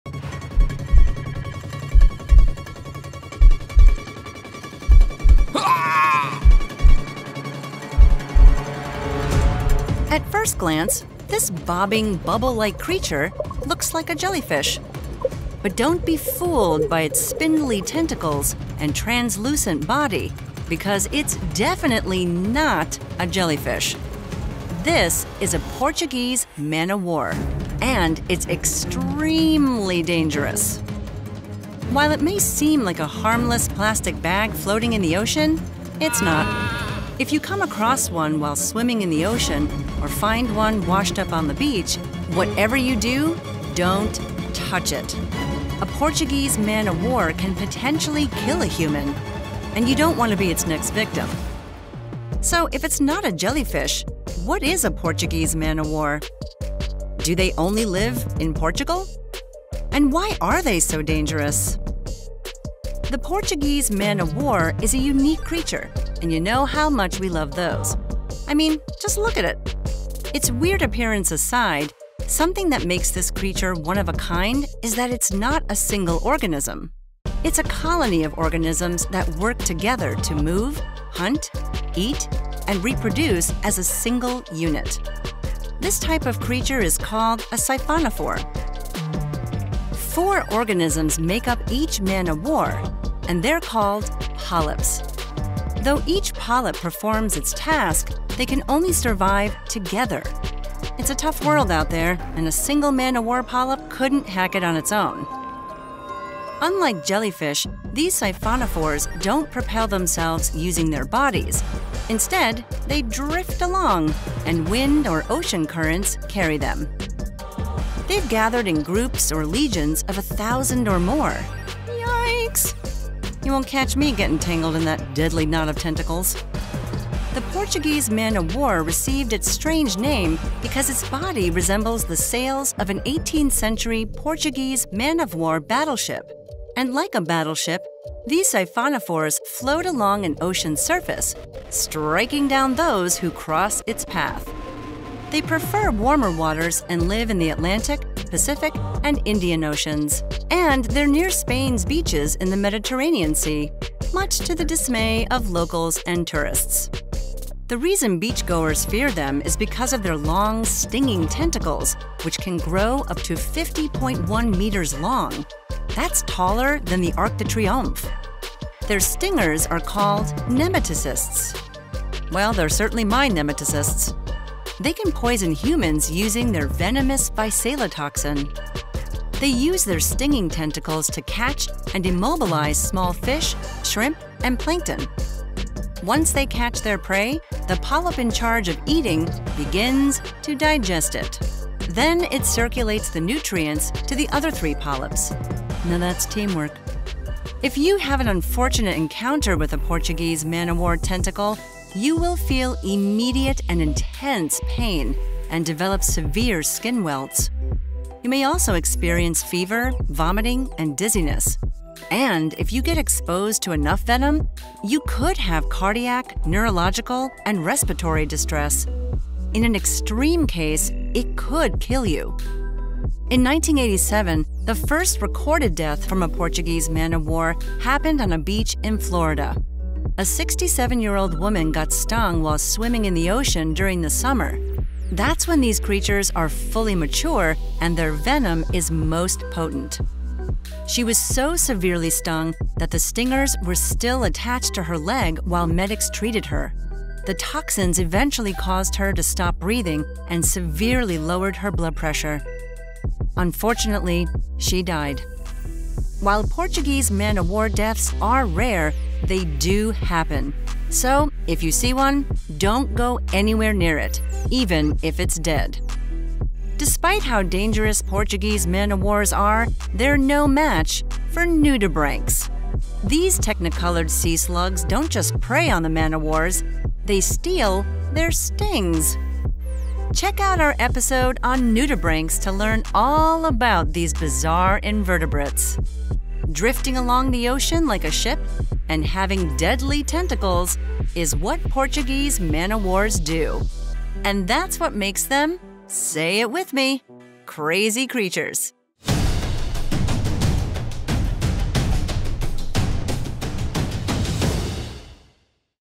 不要觸碰葡萄牙戰艦！| 瘋狂生物
第一眼看去，這隻晃來晃去，氣球那樣的東西就像一隻水母
但不要被牠細長的觸手和透明的身體騙到，它絕對不是一隻水母，而是一隻葡萄牙戰艦，而且牠極為危險